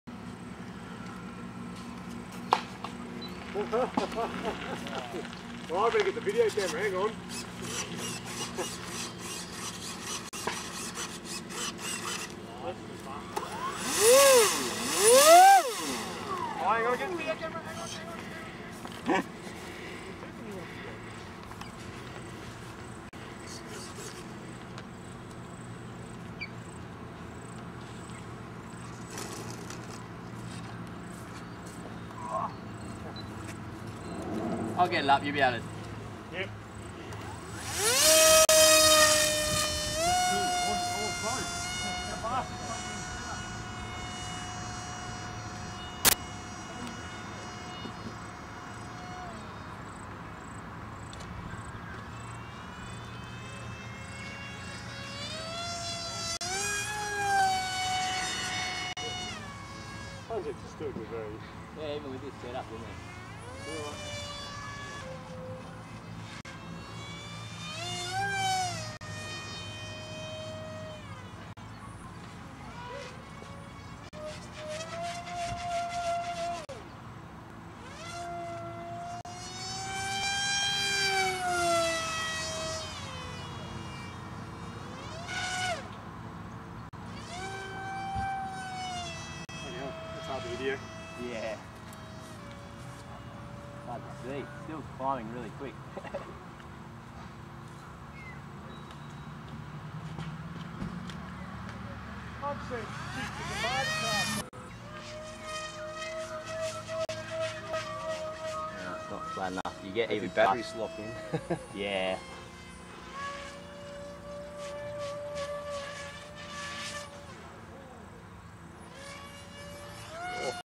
funjet thrust vectored
The new carbon fibre thrust vector mount for the fun jet works like a dream.